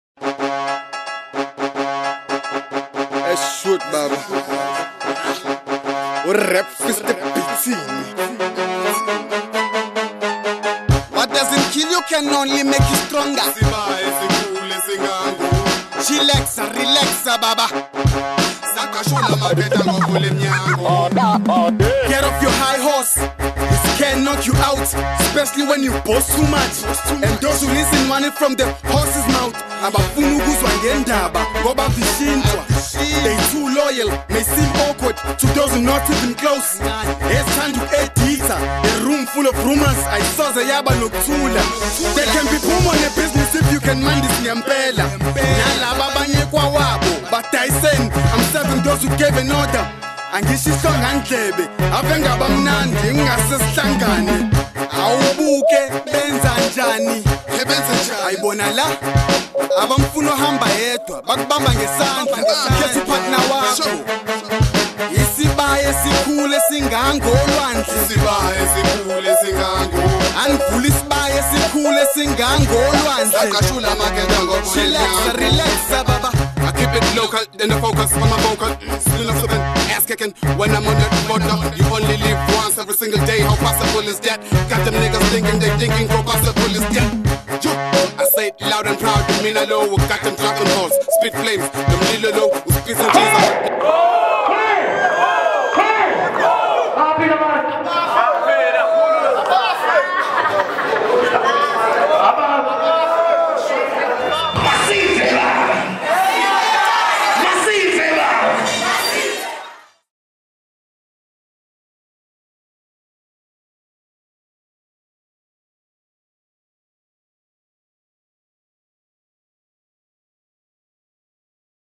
ALL BLACK PARTY
This was the first All Black Party witch was held in Margate Back Line Night club, witch featured only KZN South Coast artists.